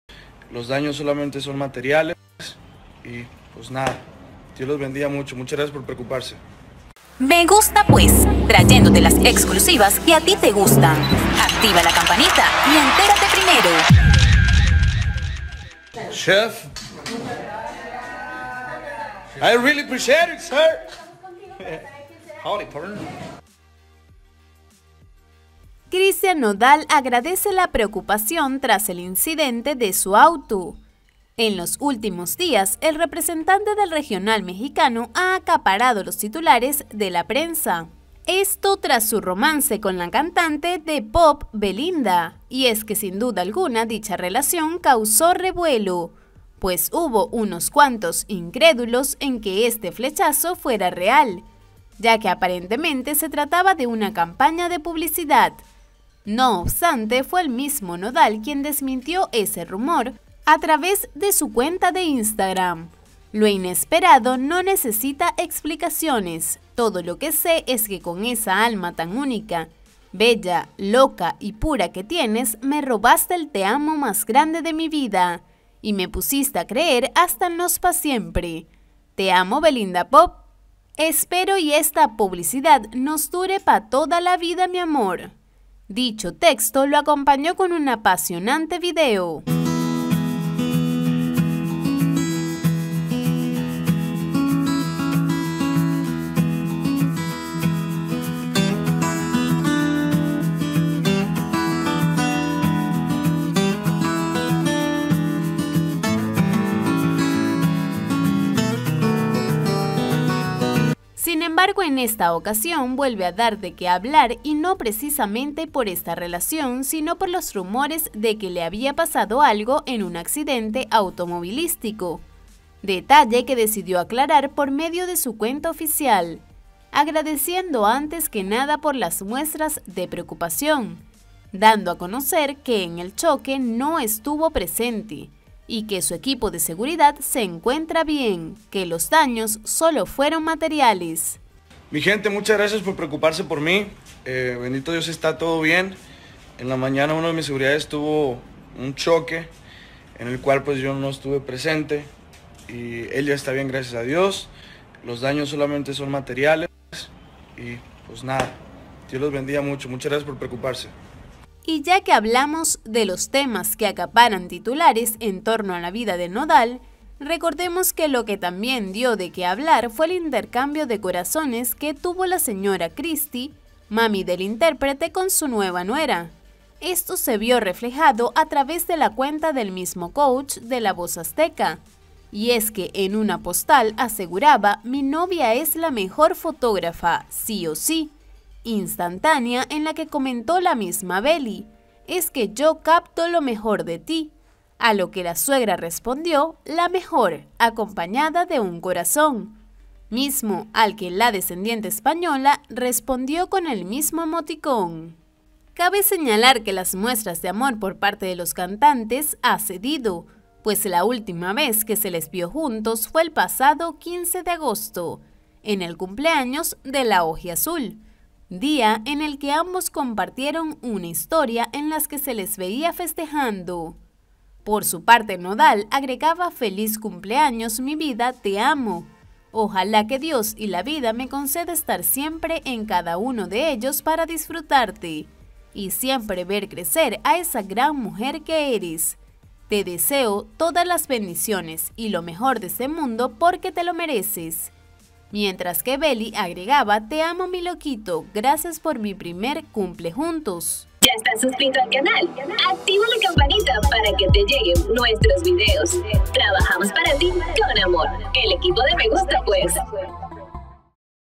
Christian Nodal confirma el choque de su auto
El cantante dio detalles del choque que tuvo su auto.